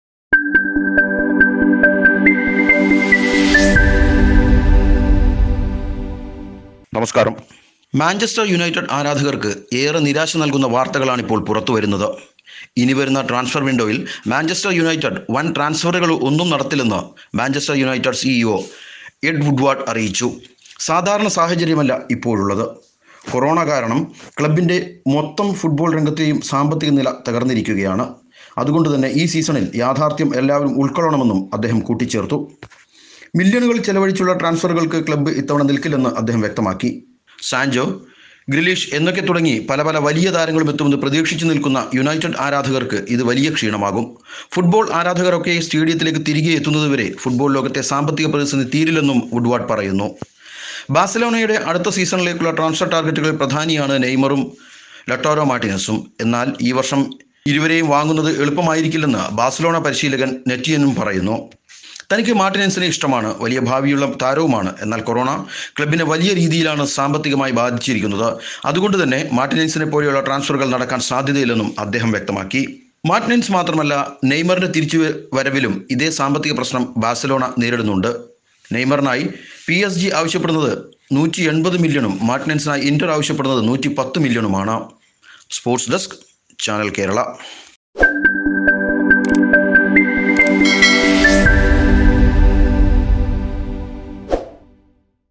ക്ലബ് ട്രാൻസ്ഫറുകൾ നടക്കില്ല, നിരാശയോടെ ആരാധകർ | Football Transfers | Manchester United | Barcelona
കൊറോണ തകർത്തു; ഫുട്ബോളിനെയും; ക്ലബ് ട്രാൻസ്ഫറുകൾ നടക്കില്ല, നിരാശയോടെ ആരാധകർ